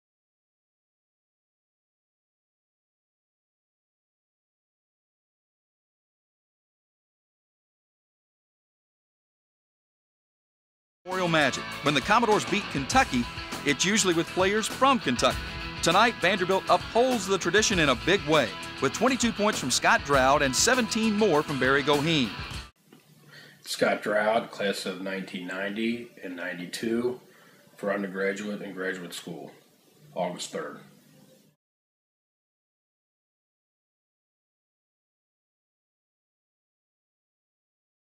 Scott Draud
Vanderbilt Commodores Men's Basketball present the 1st Annual Alumni Game.
August3rd 2013